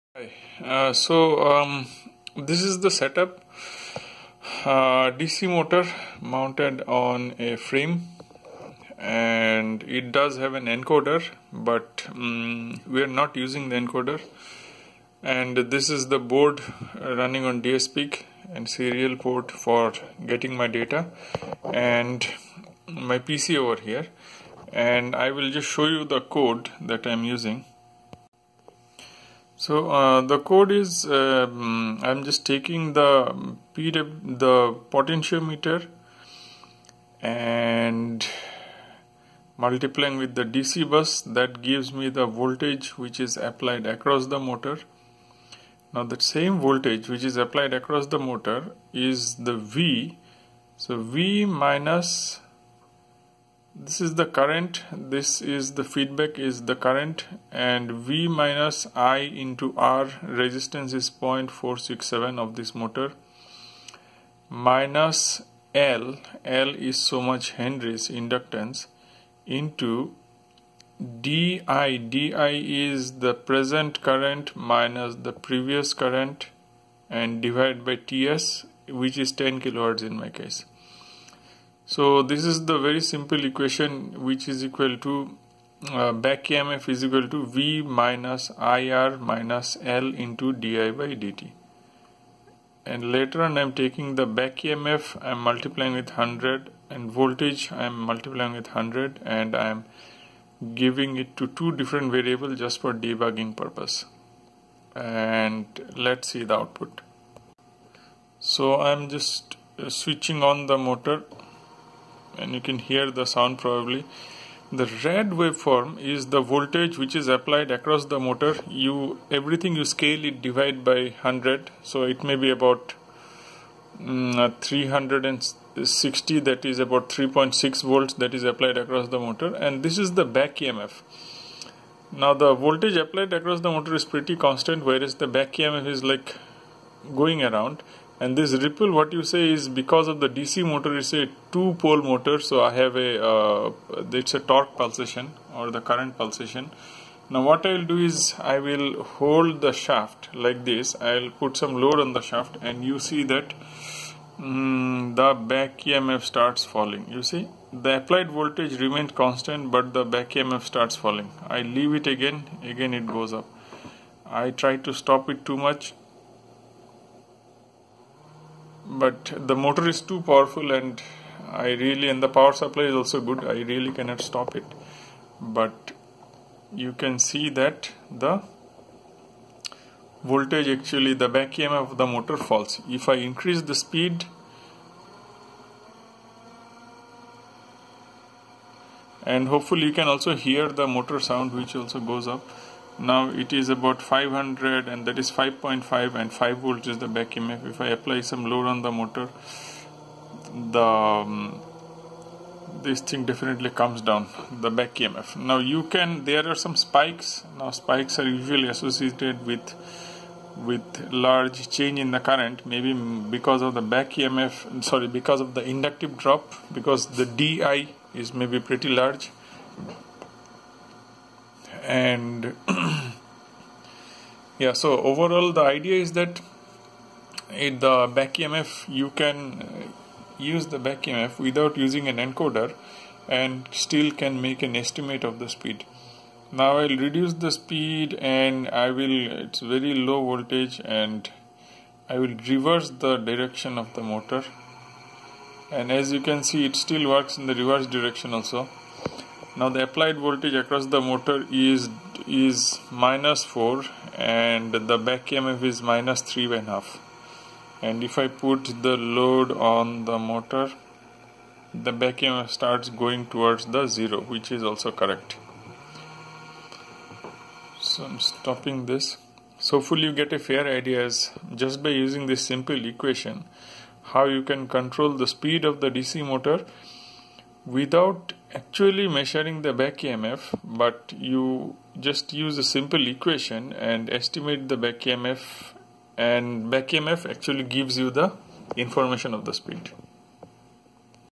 Using Back-EMF of DC motor to estimate speed.
When we want closed loop speed control of DC motor, typically encoder or other speed sensor is used. But, as shown in this demo, the back EMF of the motor can be estimated (from basic motor current), that is a scaled version of speed of motor. Once BEMF is estimated, the speed of the motor can be used as feedback to regulate the performance of motor in closed loop speed control.